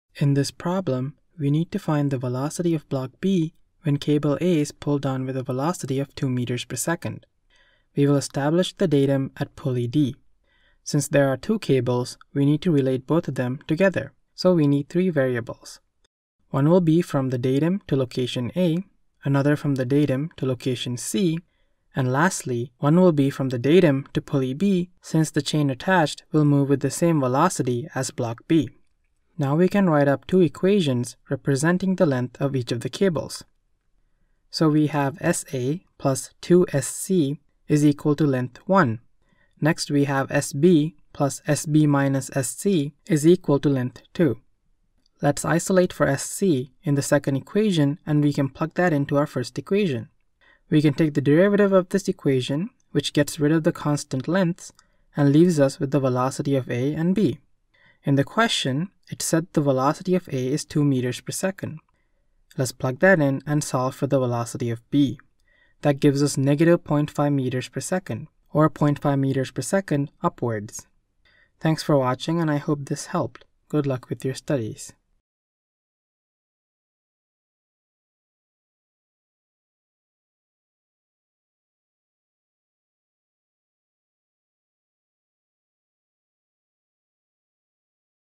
If the end of the cable at A is pulled down (solved)
If the end of the cable at A is pulled down with a speed of 2 m/s, determine the speed at which block B rises.